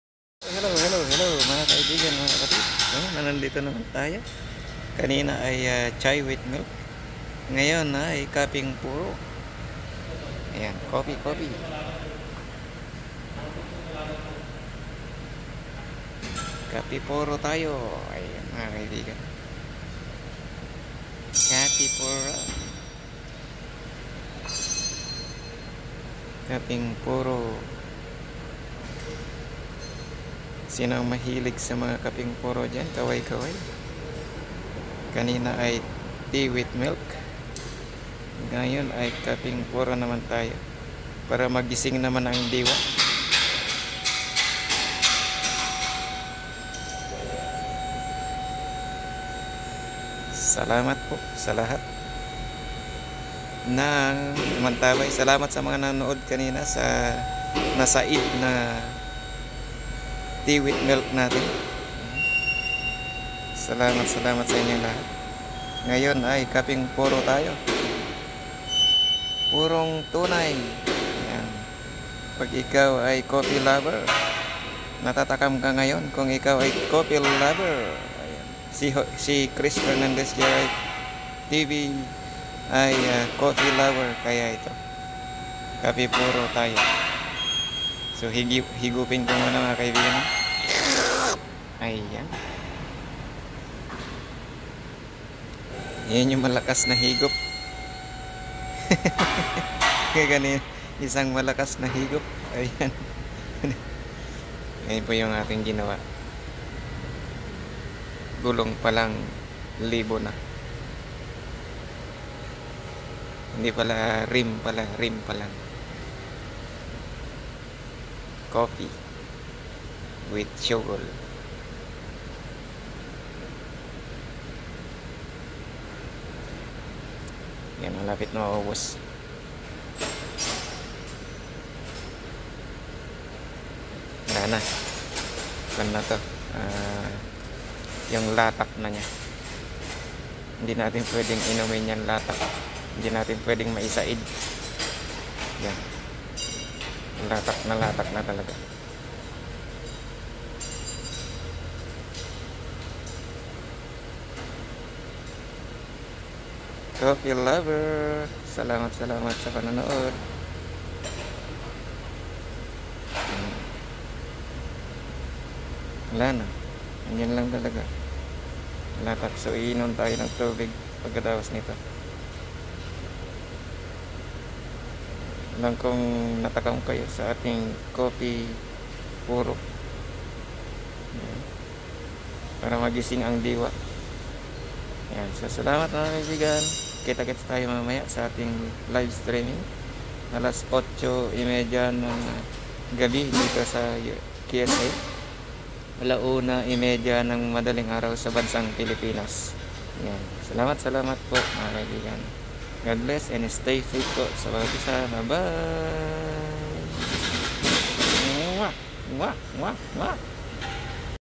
"PURE COFFEE SA TAGLAMIG NA PANAHON"
Pure coffee pampagising ng diwa kung ganito na taglamig.
Godbless po sa lahat